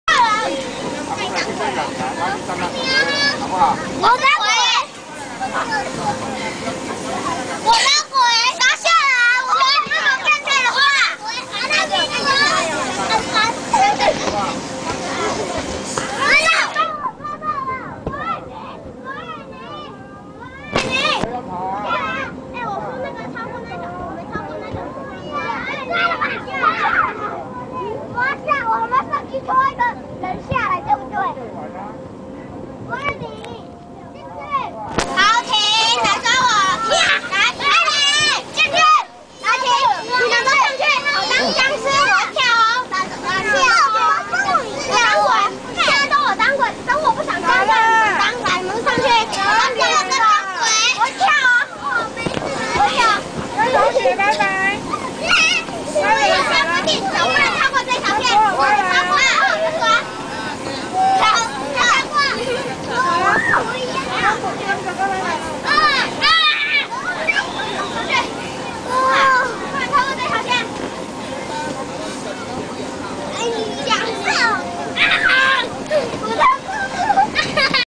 101.12.7到仁武夜市玩~大型氣模-4
妞妞和小朋友一起跌下~~